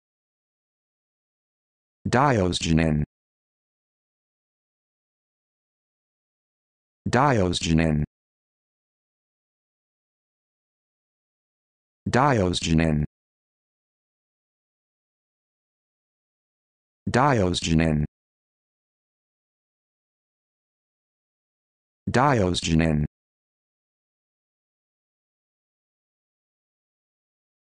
How to Pronounce Diosgenin
This video shows you how to pronounce Diosgenin